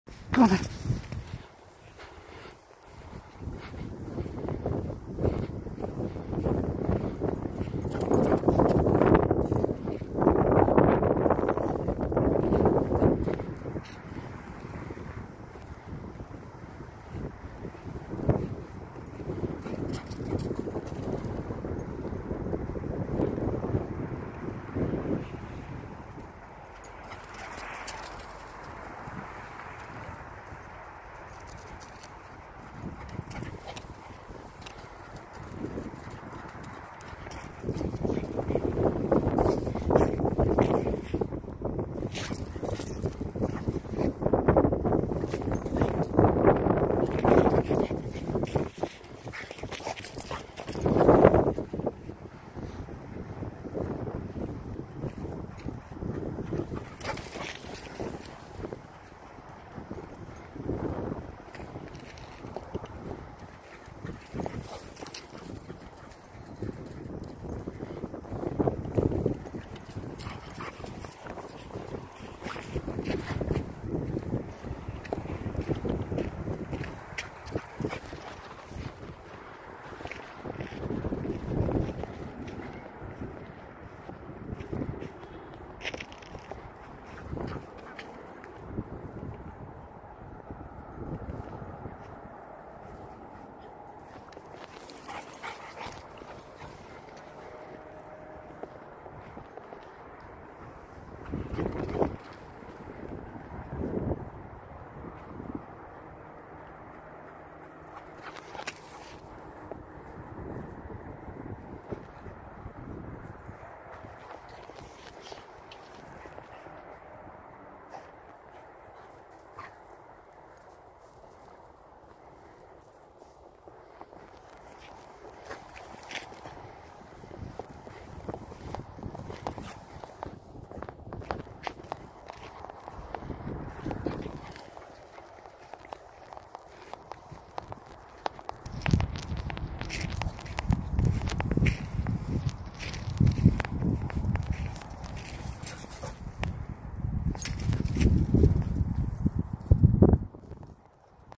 ZOOMIES in the snow 😀🌨️❄️😀🌨️❄️
We eventually got some snow at the weekend and Boris was very excited about it. Unfortunately I wasn't well enough to go out in it, so my partner kindly took Boris out to video him in the snow for me. Boris had a fine old time doing zoomies, I regret not being well enough to join in as the snow has almost disappeared now 😢 I love it when Boris has a fine old time enjoying himself like this 😁